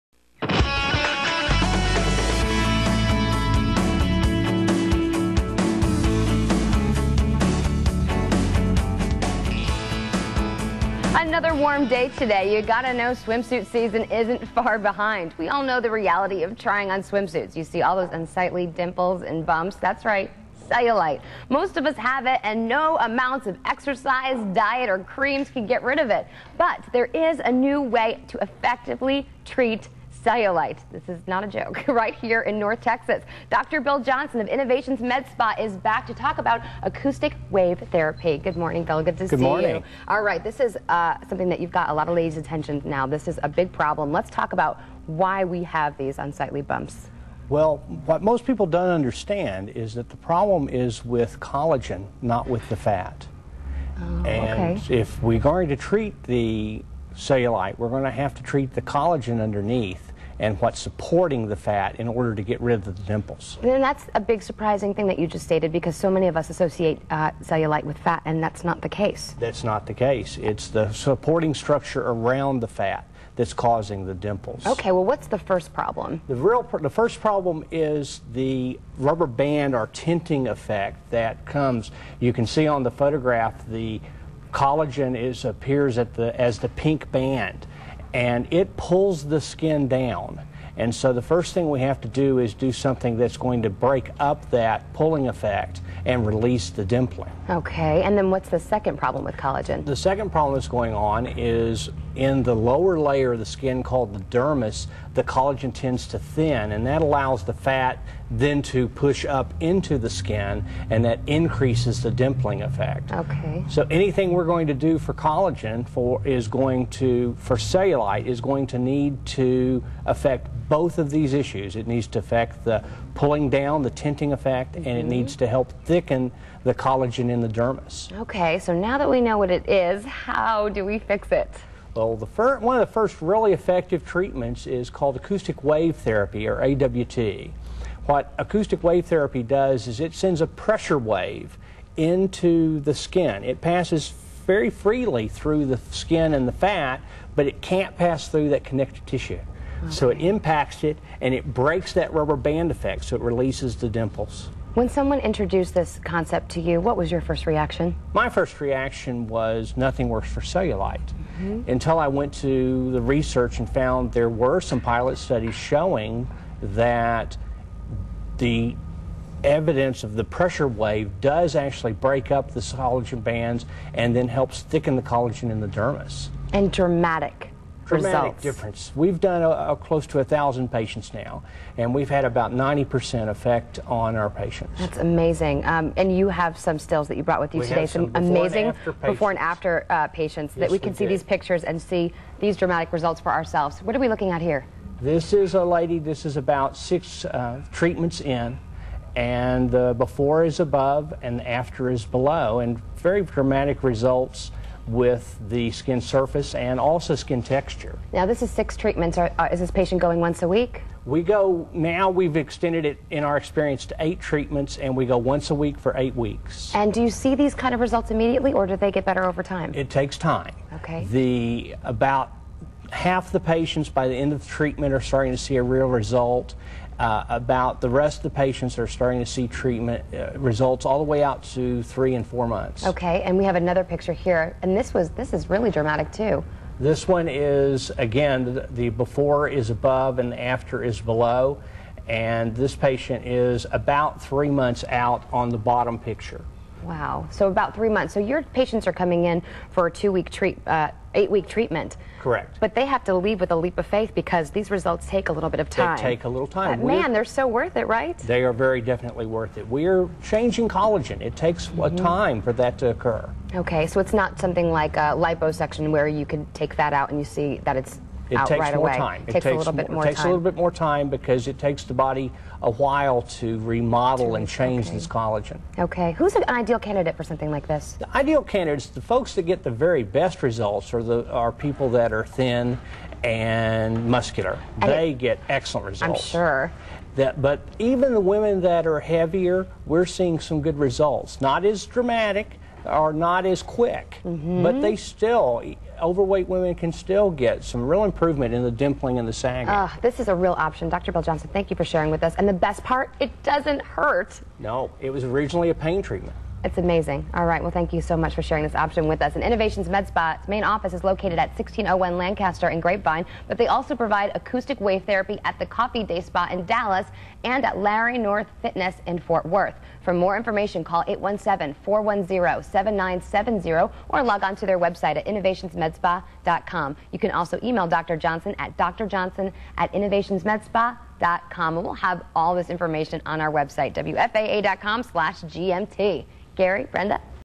What Causes Cellulite?
Dr. Bill Johnson of Innovations Medical in Dallas, Tx.  What causes stubborn cellulite and how to get rid of it!  Acoustic Wave Therapy, Thermage
Innovations Medspa